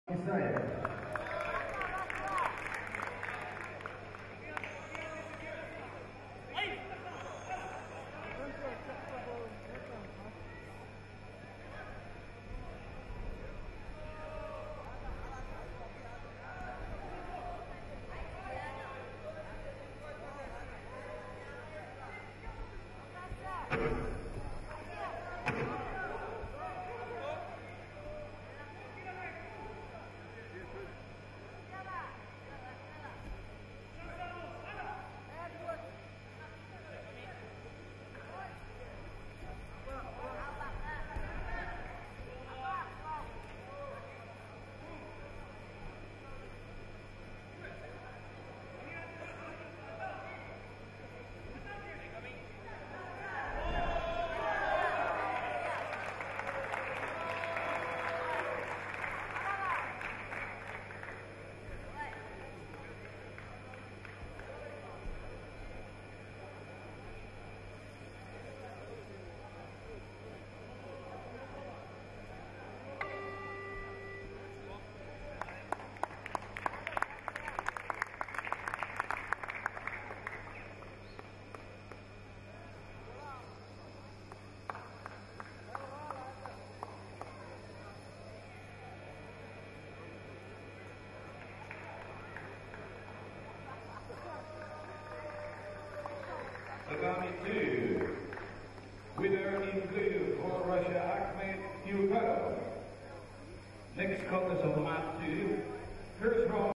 50 kg Mariniuc E Исаев А 22 09 2015 Т1 Всероссийские соревнования памяти Т А З, Кадырова, ГРОЗНЫЙ
Международный турнир памяти Т-А.З. Кадырова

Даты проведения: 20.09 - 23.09
Место проведения: Россия, Грозный
Возрастная категория: юноши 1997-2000 г.р.